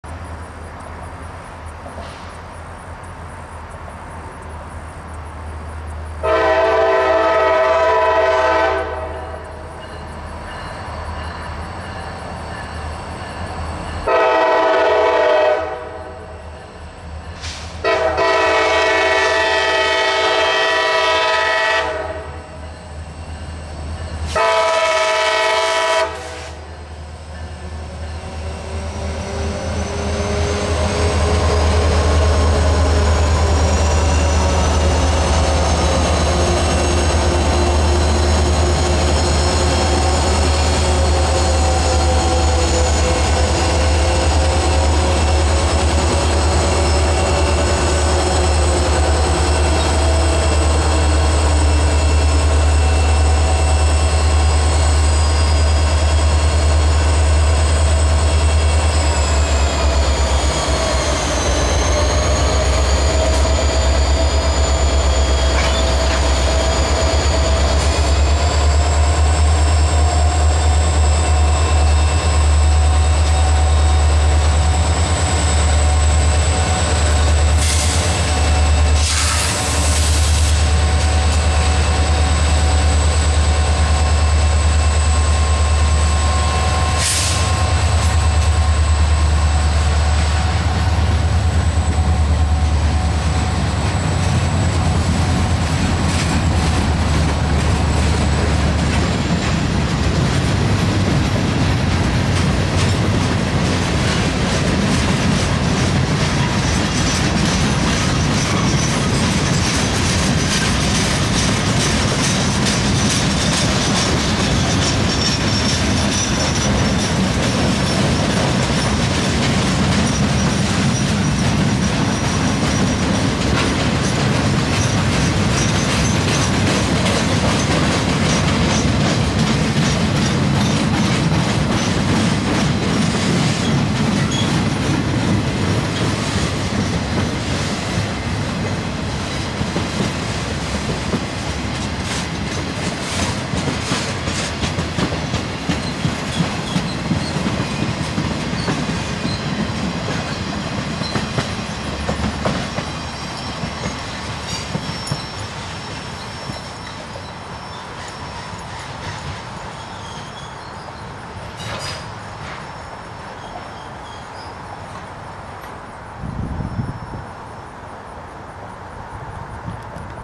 CSX SD40-3 4289 Leads MOW Train L446-16 on 8/16/22
Local L446-16 slowly heads south down the Chattanooga Sub with a MOW train set. They would drop this at Pillsbury, but I'm not sure where the specific destination is, and which job is supposed to take it.
Filmed at W Main St. in Murfreesboro, TN.

CSX 4289 (ex UP SD40 3037)- SD40-3 (Raised Letter K5LA)
CSX 8064 (ex L&N)- SD40-2
CSX 4070 (ex L&N SD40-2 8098)- SD40-3